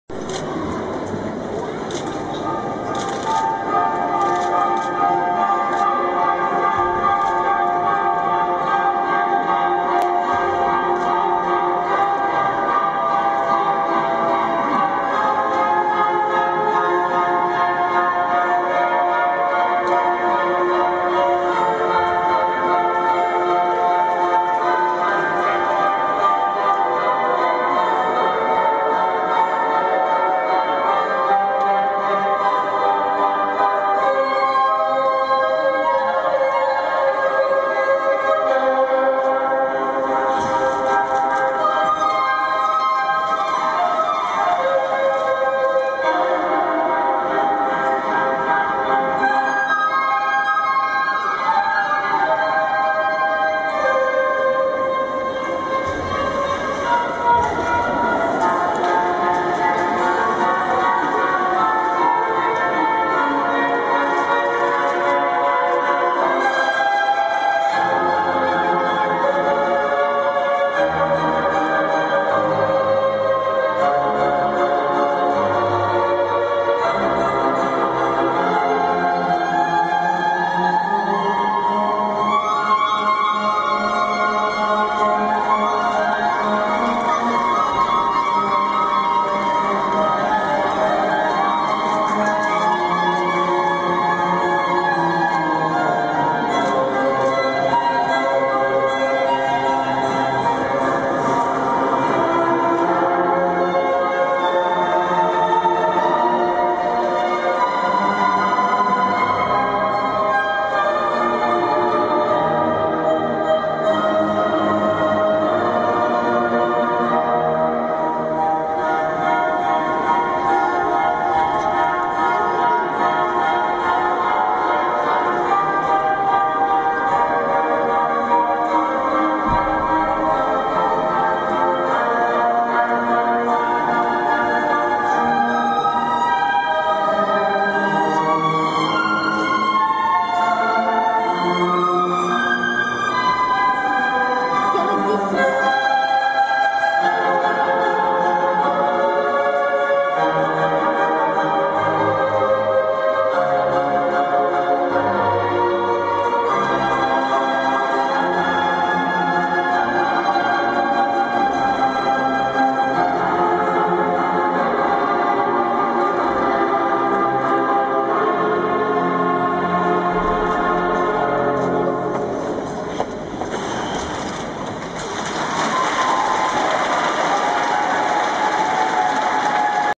Мартюшева-Рогонов, короткая программа
Открытые прокаты сборной России, Москва, 7 сентября 2011